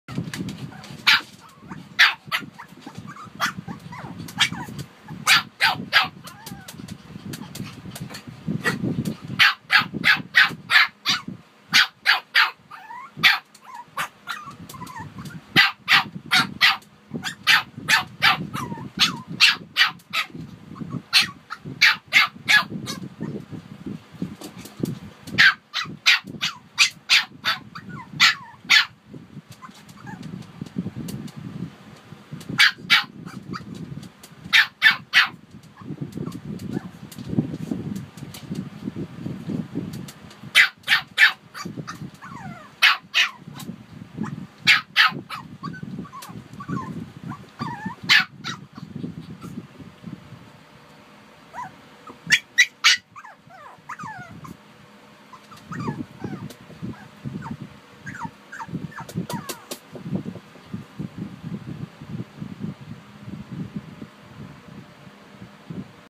Shih Tzu Barking - Harper and Wuhan
Our Baby Wuhan with our "ampon" for 4 days Baby Harper.